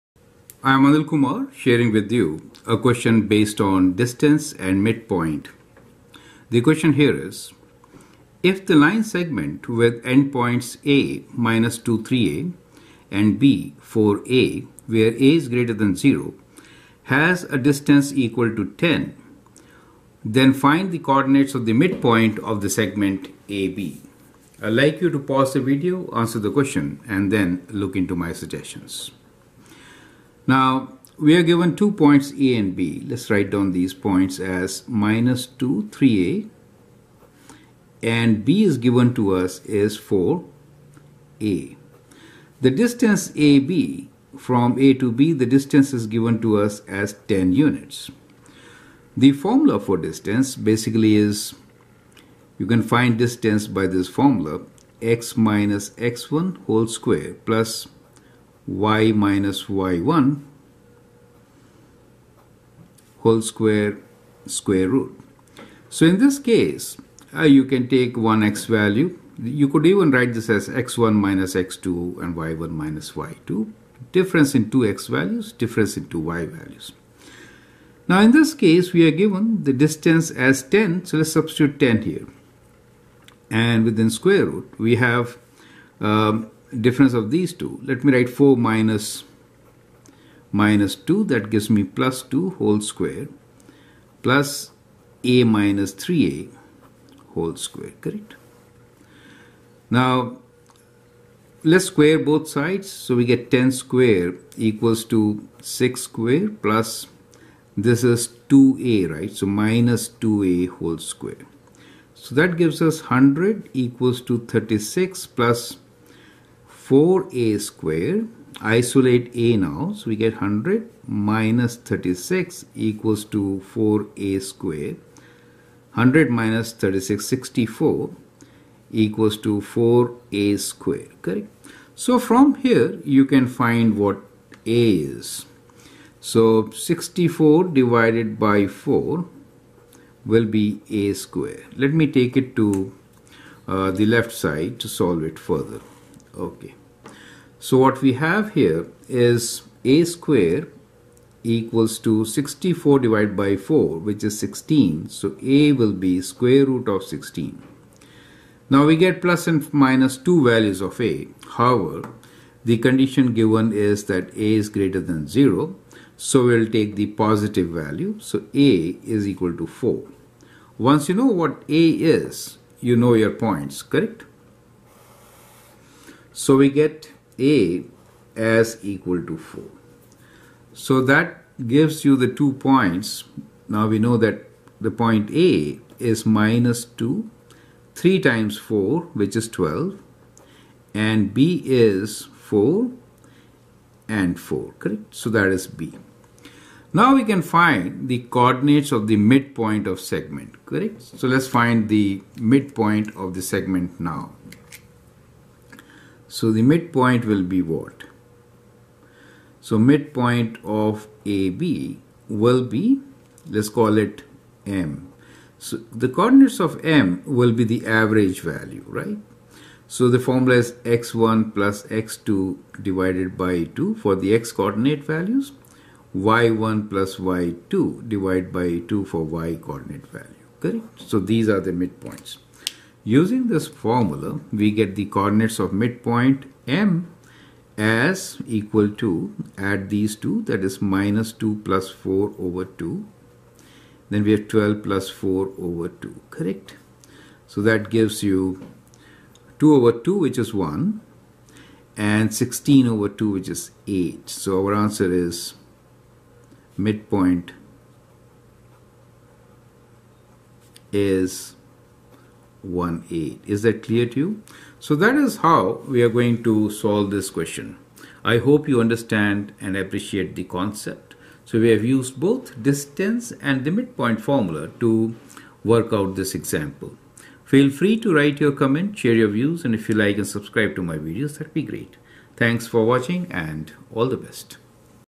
Find mid point of 10 unit segment A (2, 3a) B (4, a) Coordinate Geometry
If the line segment with endpoints A(-2, 3a) and B(4, a), has a distance equal to 10, then find the coordinates of the midpoint of the segment AB.